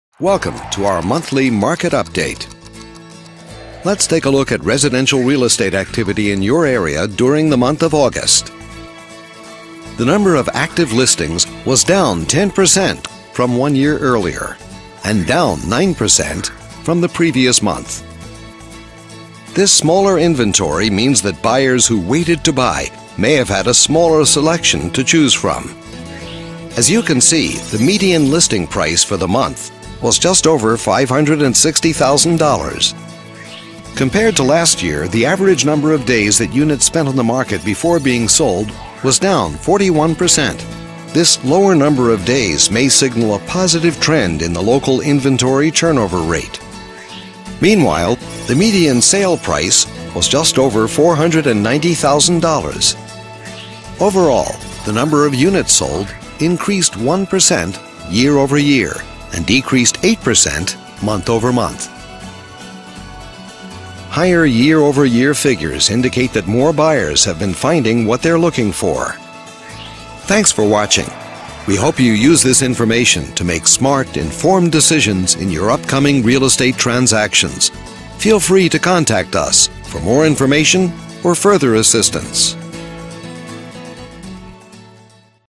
Fairfax City Merrifield,VA, Real Estate Market Update from NRT DC-Baltimore,August,2013
NRT DC-Baltimore Video Update recapping market activity within Fairfax City Merrifield, VA, during the month of August, 2013